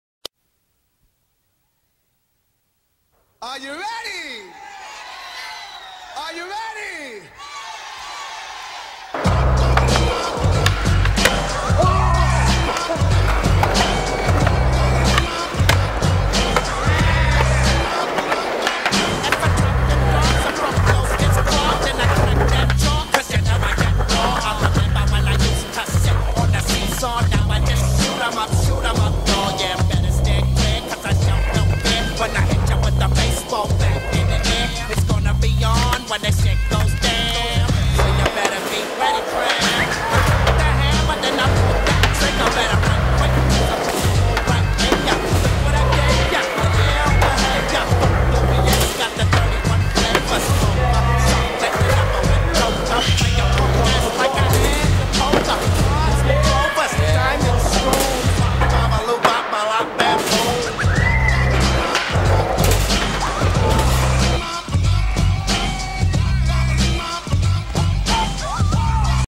Quick Clips
Skateboarders: Sam Mueller, Carlos Cardoza, Jake Szabo, Gustavo Gonzalez, and Manny Cruz

living in wisconsin sucks especially during winter because you can't street skate